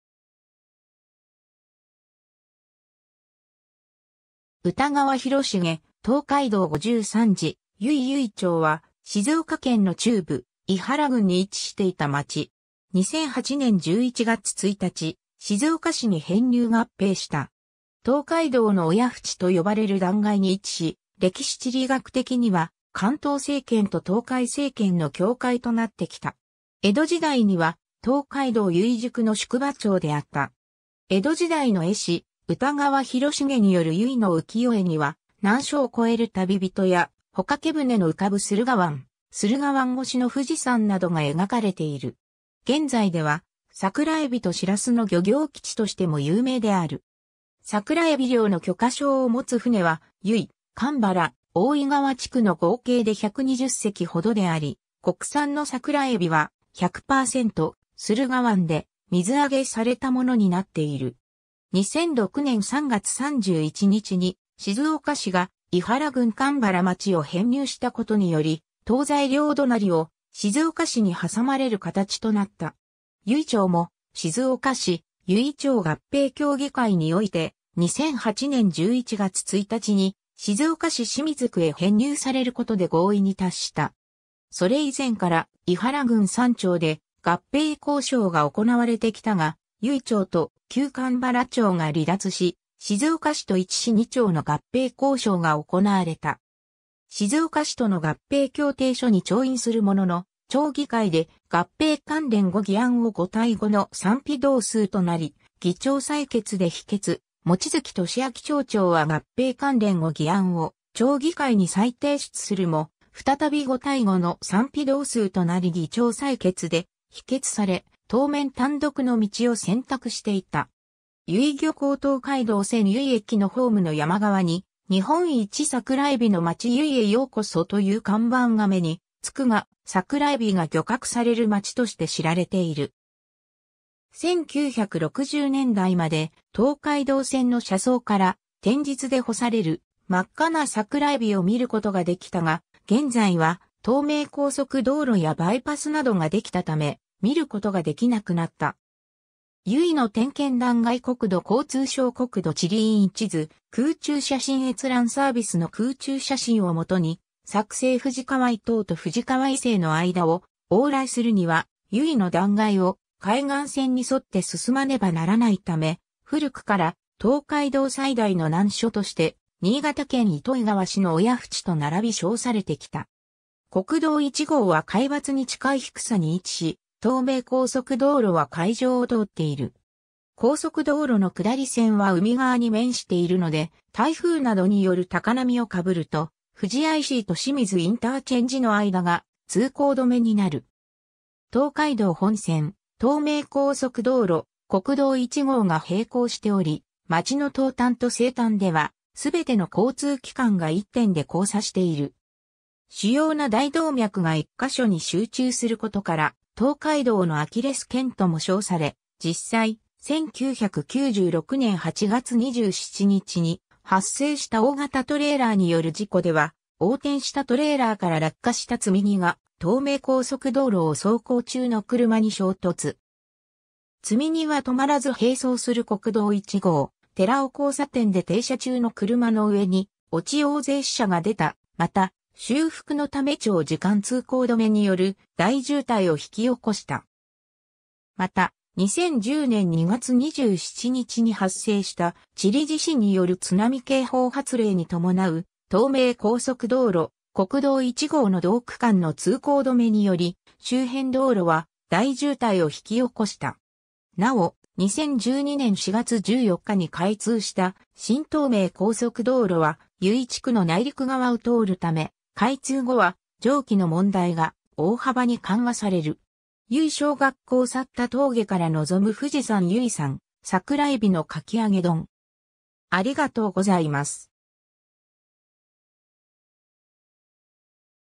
由比町
歌川広重「東海道五十三次・由比」 由比町（ゆいちょう）は、静岡県の中部、庵原郡に位置していた町。
2008年（平成20年）11月1日、静岡市に編入合併した。
 「東海道の親不知」と呼ばれる断崖に位置し、歴史地理学的には関東政権（江戸・鎌倉・小田原）と東海政権（駿府）の境界となってきた。
 江戸時代には東海道由比宿の宿場町であった。
江戸時代の絵師・歌川広重による由比の浮世絵には、難所を越える旅人や、帆掛け船の浮かぶ駿河湾、駿河湾越しの富士山などが描かれている。
 現在では、サクラエビとシラスの漁業基地としても有名である。
 桜えび漁の許可証をもつ船は、由比・蒲原・大井川地区の合計で 120隻ほどであり、国産の桜えびは、100%駿河湾で水揚げされたものになっている。
 2006年3月31日に静岡市が庵原郡蒲原町を編入したことにより、東西両隣を静岡市に挟まれる形となった。
由比町も「静岡市・由比町合併協議会」において、2008年11月1日に静岡市清水区へ編入されることで合意に達した。
それ以前から庵原郡三町（由比町、富士川町、旧蒲原町）で合併交渉が行われてきたが、由比町と旧蒲原町が離脱し、静岡市と1市2町の合併交渉が行われた。
静岡市との合併協定書に調印するものの、町議会で合併関連5議案を5対5の賛否同数となり議長（安部庄太郎議長）裁決で否決、望月俊明町長は合併関連5議案を町議会に再提出するも、再び5対5の賛否同数となり議長裁決で否決され、当面単独の道を選択していた。
 由比漁港 東海道線由比駅のホームの山側に「日本一桜えびのまち由比へようこそ」という看板が目につくが、サクラエビが漁獲される町として知られている。
1960年代まで、東海道線の車窓から、天日で干される真っ赤な桜えびを見ることができたが、現在は東名高速道路やバイパスなどができたため、見ることができなくなった。
 由比の天険断崖 国土交通省 国土地理院 地図・空中写真閲覧サービスの空中写真を基に作成 富士川以東と富士川以西の間を往来するには、由比の断崖を海岸線に沿って進まねばならないため、古くから東海道最大の難所として、新潟県糸魚川市の親不知と列び称されてきた。
国道1号は海抜に近い低さに位置し、東名高速道路は海上を通っている。
高速道路の下り線は海側に面しているので、台風などによる高波を被ると富士ICと清水ICの間（波が高い場合は上下線とも）が通行止めになる。
東海道本線・東名高速道路・国道1号が並行しており、町の東端と西端ではすべての交通機関（この区間をトンネルで貫く東海道新幹線を除く）が一点で交差している。
主要な大動脈が一箇所に集中することから東海道のアキレス腱とも称され、実際、1996年（平成8年）8月27日に発生した大型トレーラーによる事故では、横転したトレーラーから落下した積荷が東名高速道路を走行中の車に衝突。
積み荷は止まらず並走する国道1号寺尾交差点で停車中の車の上に落ち大勢死者が出た・また、修復のため長時間通行止めによる大渋滞を引き起こした。
 また、2010年2月27日に発生したチリ地震による津波警報発令に伴う東名高速道路・国道1号の同区間の通行止めにより、周辺道路は大渋滞を引き起こした。
 なお、2012年4月14日に開通した新東名高速道路は、由比地区の内陸側を通るため、開通後は上記の問題が大幅に緩和される。
 由比小学校 薩埵峠から望む富士山 由比産サクラエビのかき揚げ丼